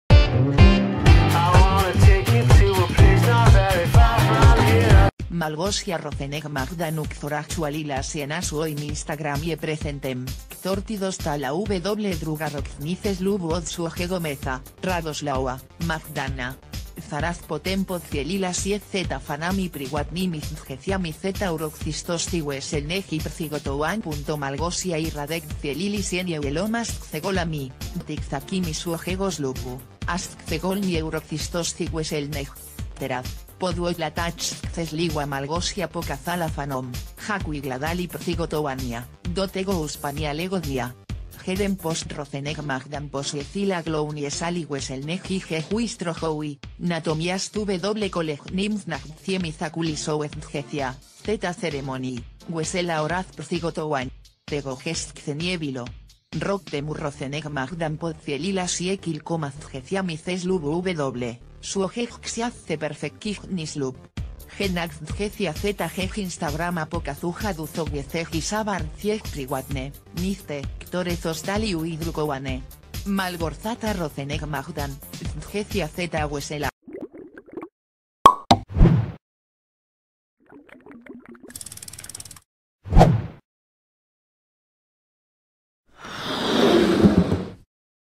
Rozenek-Majdan w rocznicę ślubu pokazała mnóstwo zdjęć z wesela. Co za dekoracje! Są nawet zapros...
Rozenek-Majdan w rocznicę ślubu pokazała mnóstwo zdjęć z wesela. Co za dekoracje! Są nawet zaproszenia i zastawa stołowa